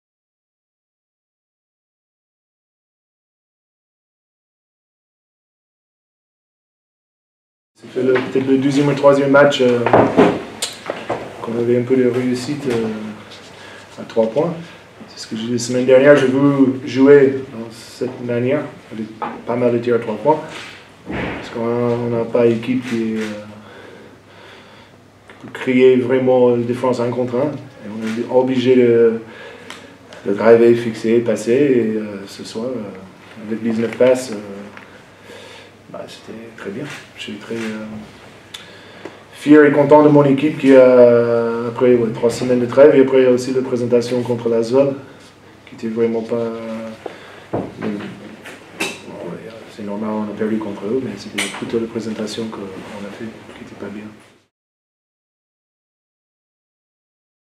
CB -Kyle Milling après Cholet - Hyères-Toulon (14-01-17)
Les réactions de Kyle Milling après Cholet - Hyères-Toulon (ProA du 14-01-17).